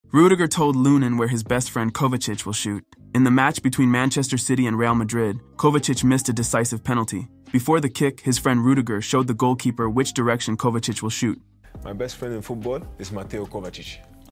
Rudiger told Lunin where his friend Kovacic will shoot😮
Rudiger told Lunin where his best friend Kovacic will shoot.